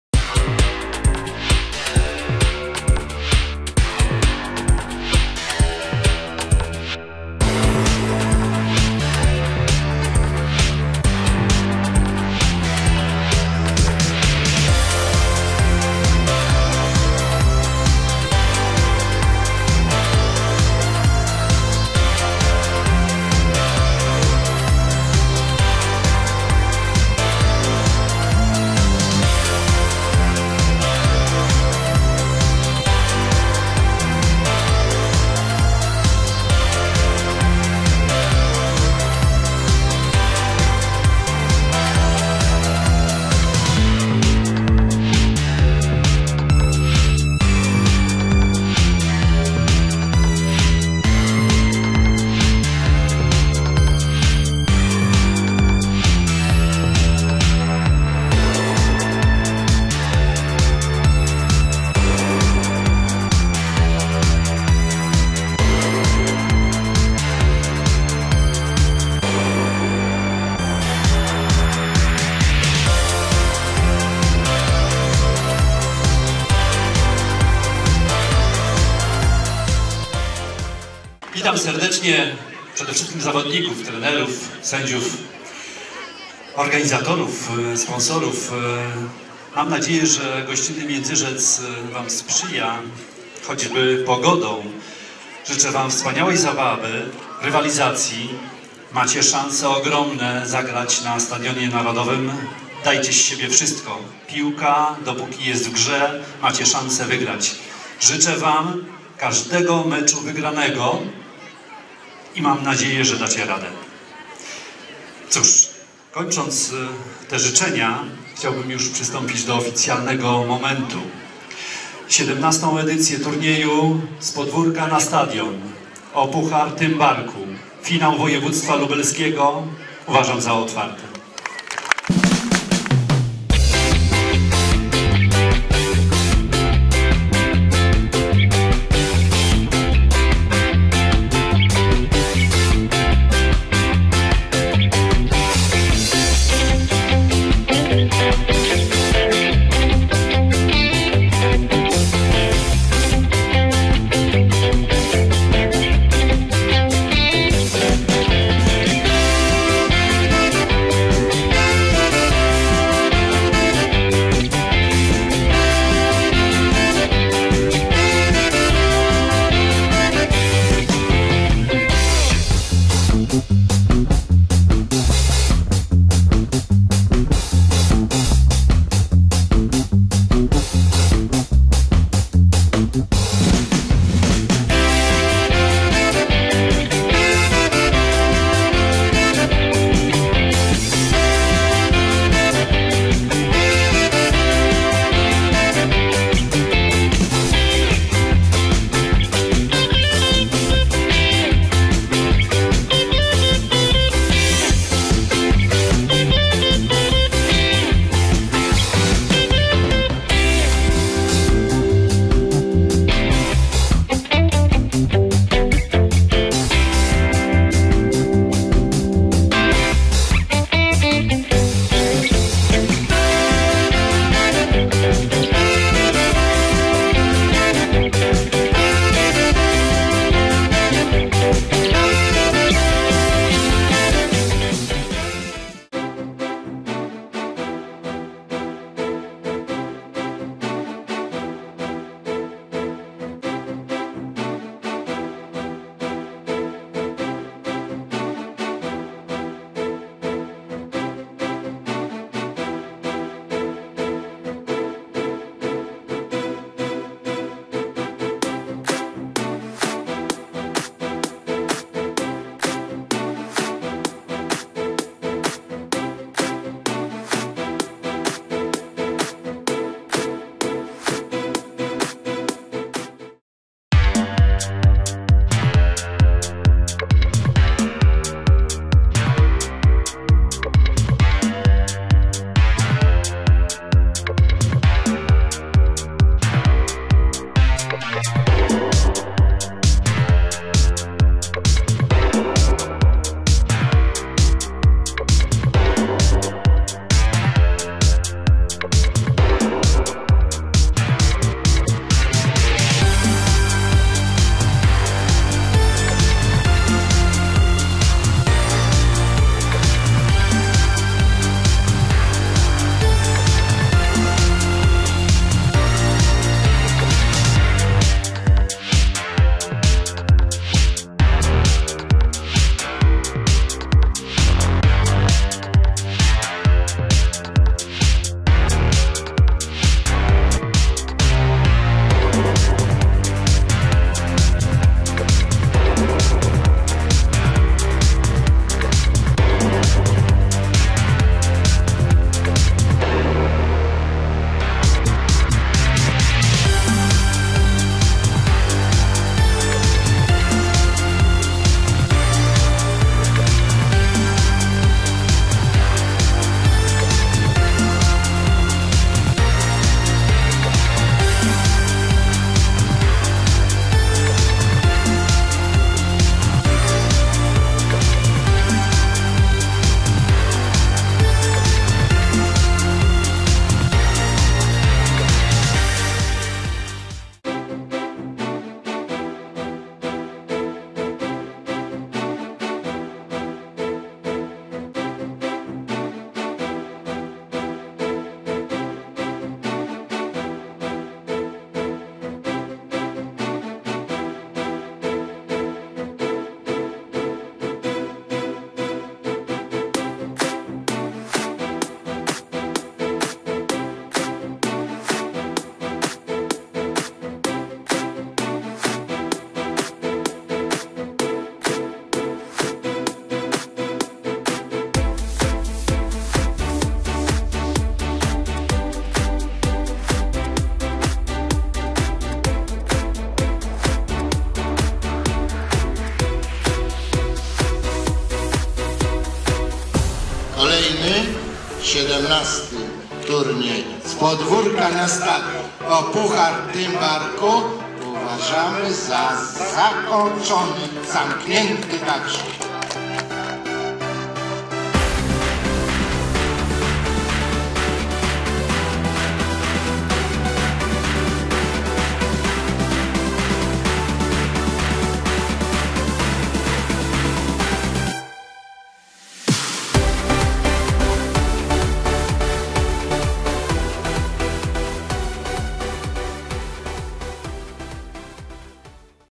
Turniej "Z Podwórka na Stadion o Puchar Tymbarku" dzień 2
Finał Województwa Lubelskiego Turnieju "Z Podwórka na Stadion o Puchar Tymbarku" - Międzyrzec Podlaski 5-6 kwietnia 2017  (dzień drugi).